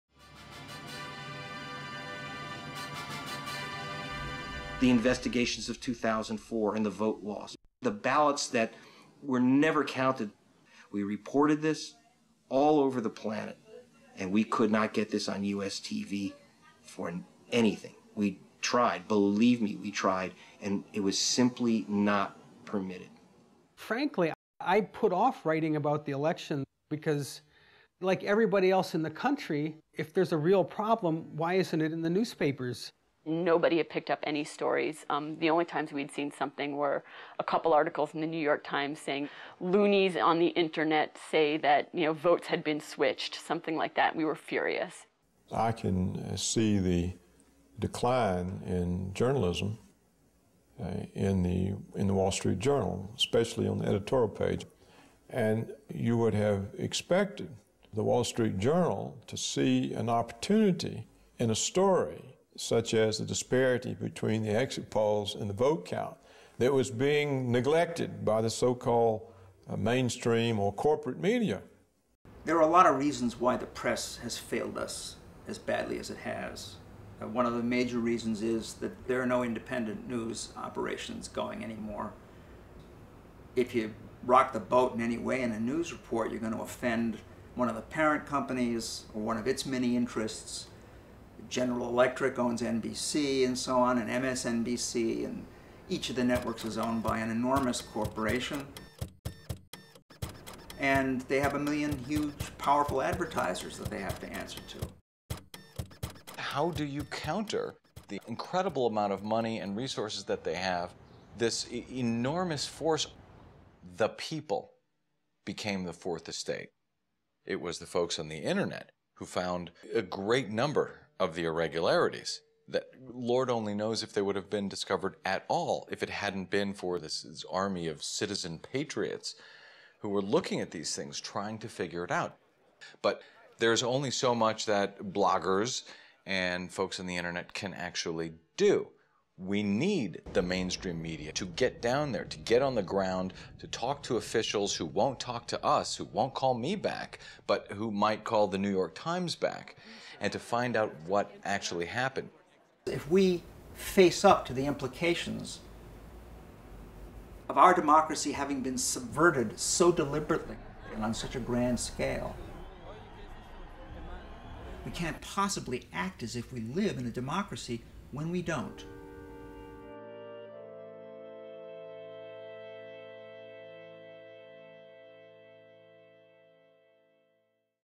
The Silence of the Media
Part of the Elections at Risk campaign. Journalists speak out about the mainstream media's failure to report election irregularities and discrepancies occurring throughout the country.